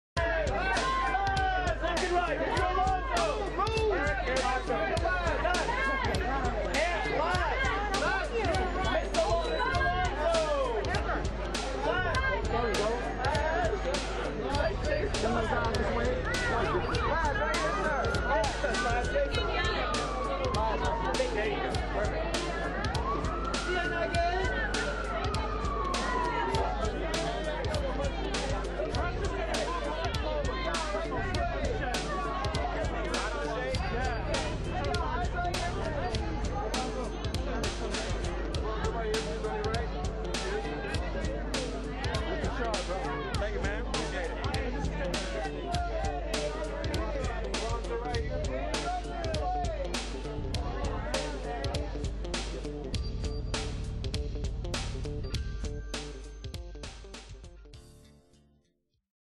LAZ ALONSO at "Jumping The Broom" LA Premiere
Laz Alonso (Breakout Kings, Avatar, Jumping The Broom) at "Jumping The Broom" at Arclight Cinerama Dome in Los Angeles, California USA May 4, 2011